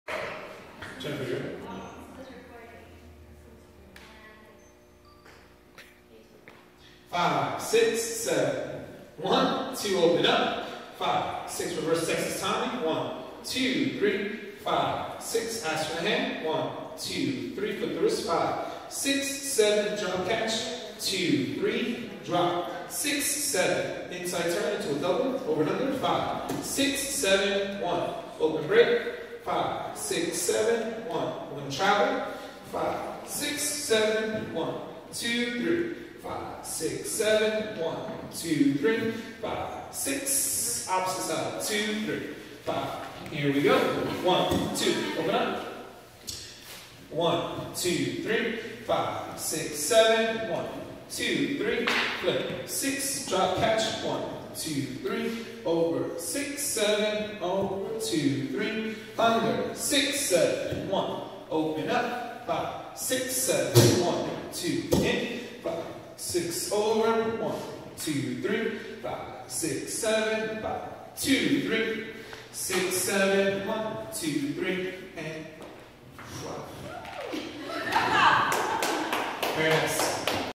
Salsa Turn Pattern - Nieves Latin Dance Studio
Wil Nieves and Kathy teaching a Level 3 turn pattern.  Visit us online for more information on dance classes.